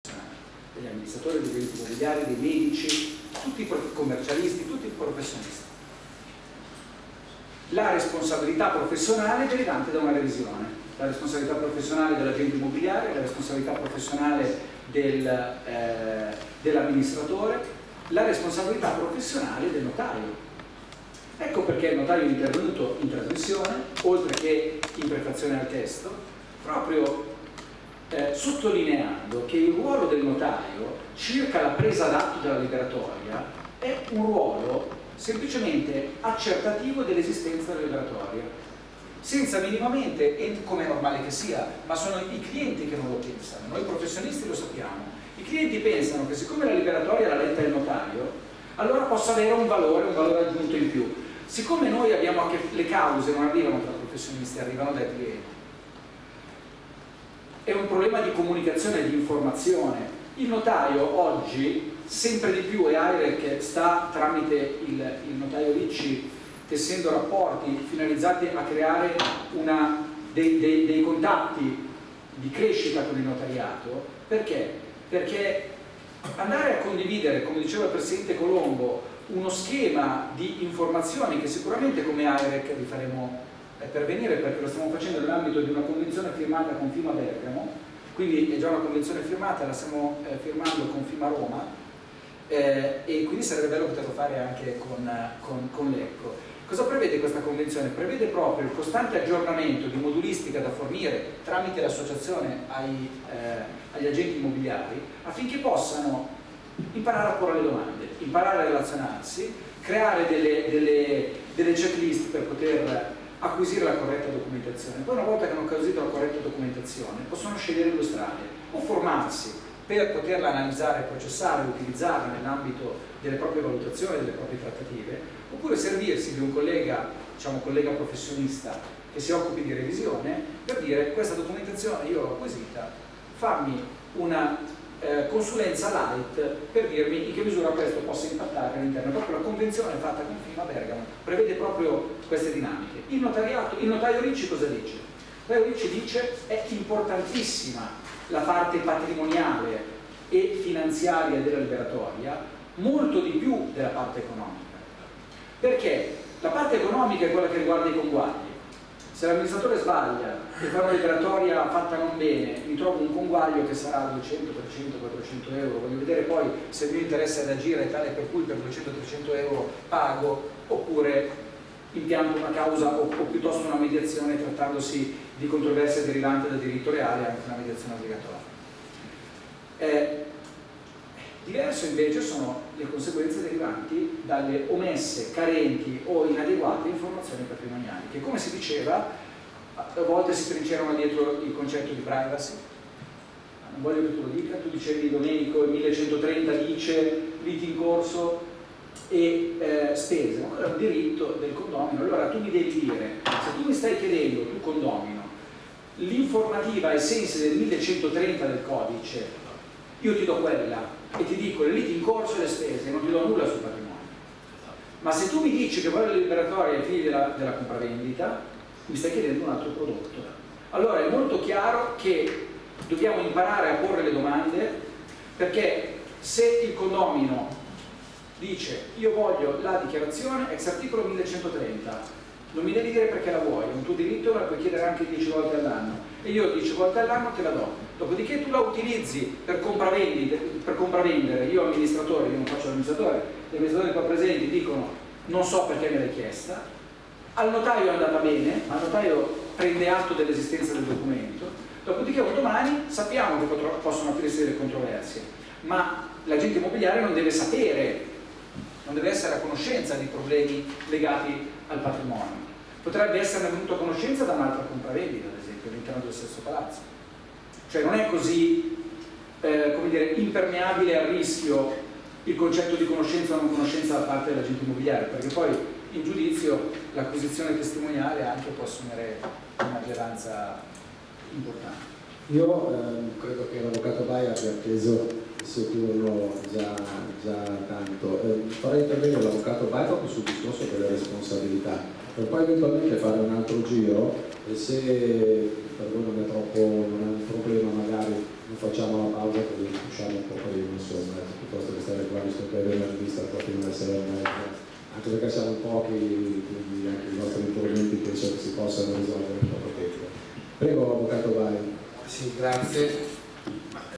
LIBERATORIA DELL’AMMINISTRATORE E COMPRAVENDITE IMMOBILIARI - 11/17
SEMINARIO
LA LIBERATORIA DELL’AMMINISTRATORE DI CONDOMINIO QUALE STRUMENTO INDISPENSABILE PER LO SVOLGIMENTO DELL’ATTIVITA’ DEGLI AGENTI IMMOBILIARI
Evento organizzato in collaborazione con AIReC, il Collegio Geometri di Lecco e FIMAA Lecco
venerdì 07 luglio 2017 – ore 14:15-18:15
ConfCommercio Lecco – piazza Garibaldi 4, Lecco
ingresso gratuito al convegno, riconoscimento n.2 crediti Geometri, n.5 crediti ICAF e AIReC